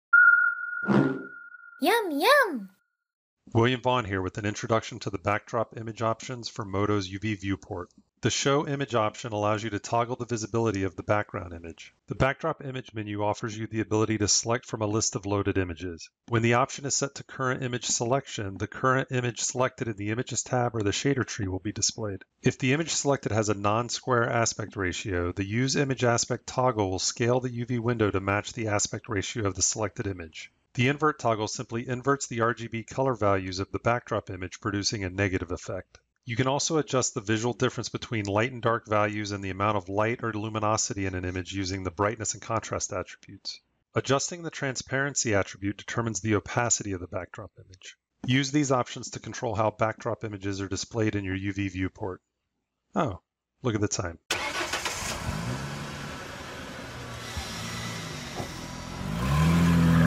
MODO | UV Viewport: Backdrop Image Options
This video is an introduction to the Backdrop Image Options for MODO's UV Viewport.